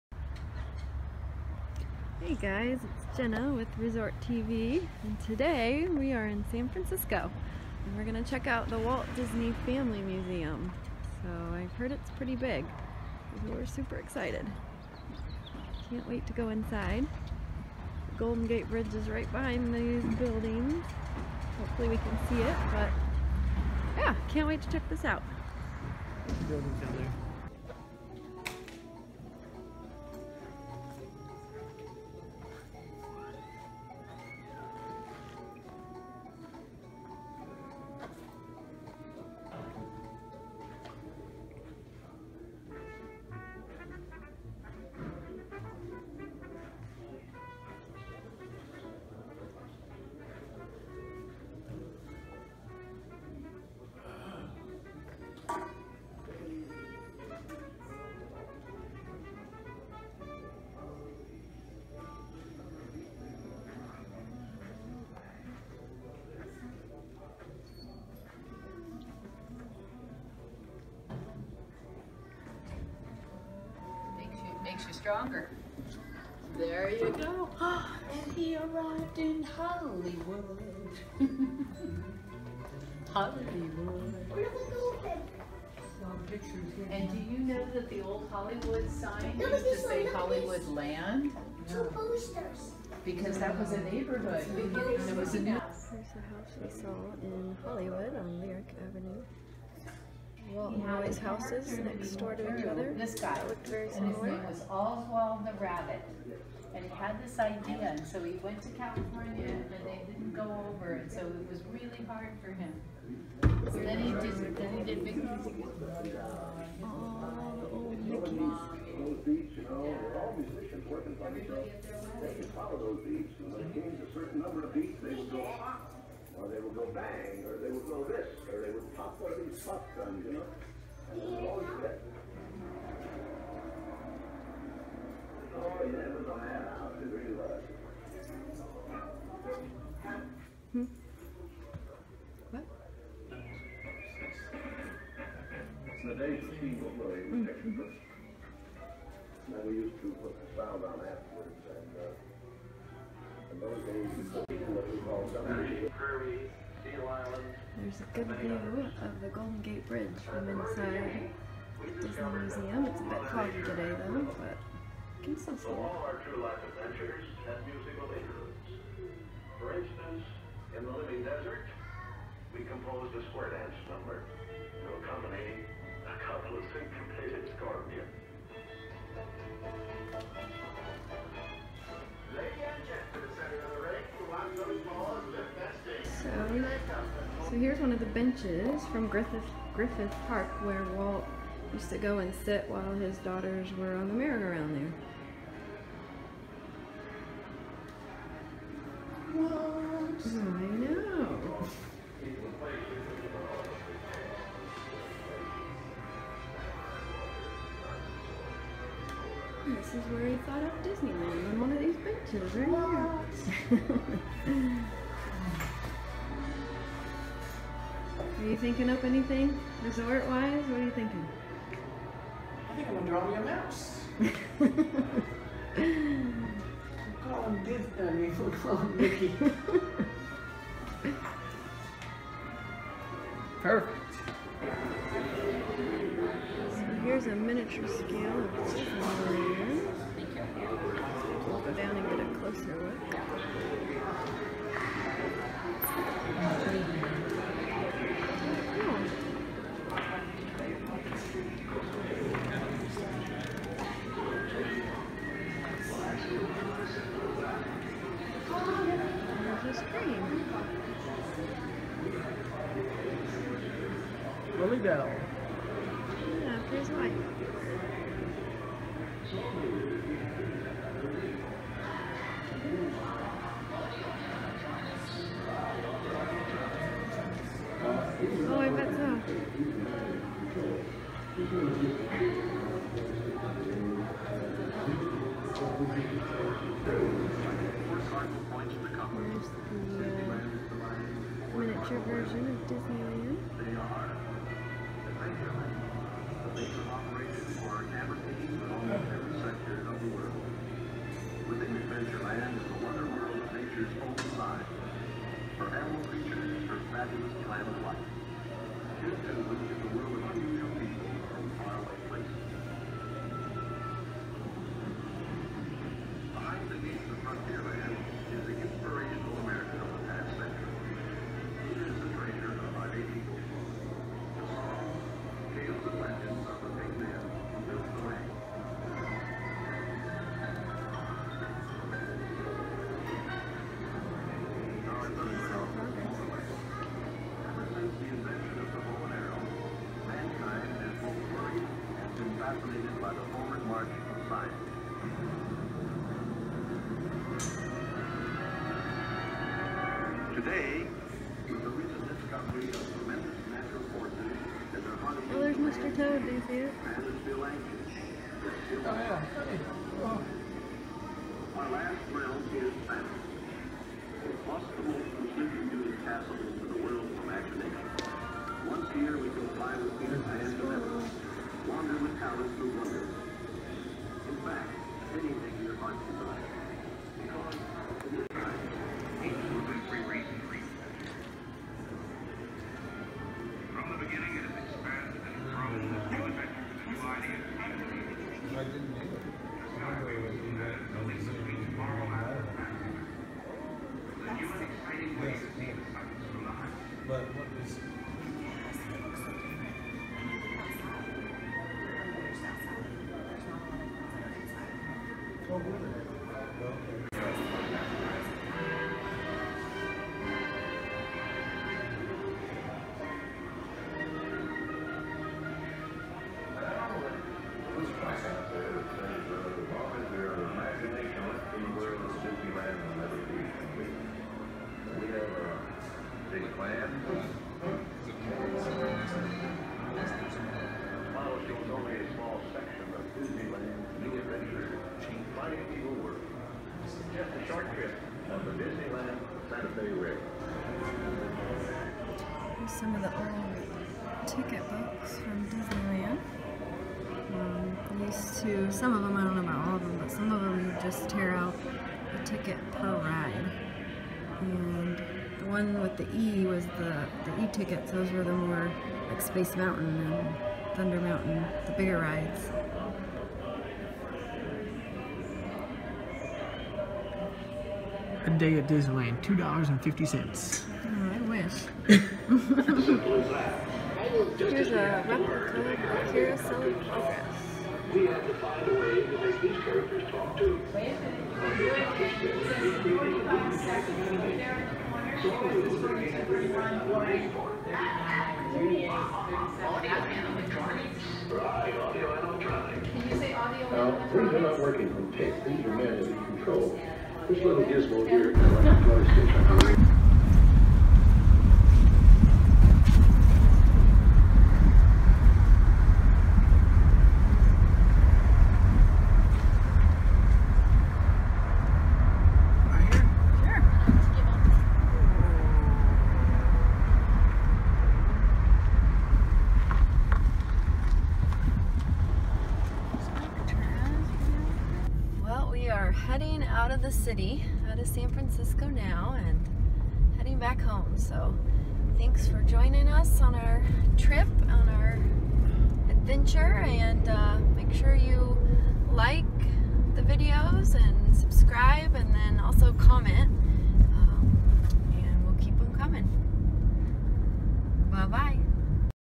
Walt Disney Family Museum | Walkthrough Tour | San Francisco
Join our guest hosts, Jenna and Dale, as they tour the Walt Disney Family Museum in San Francisco.  They also get to drive by the amazing Golden Gate Bridge!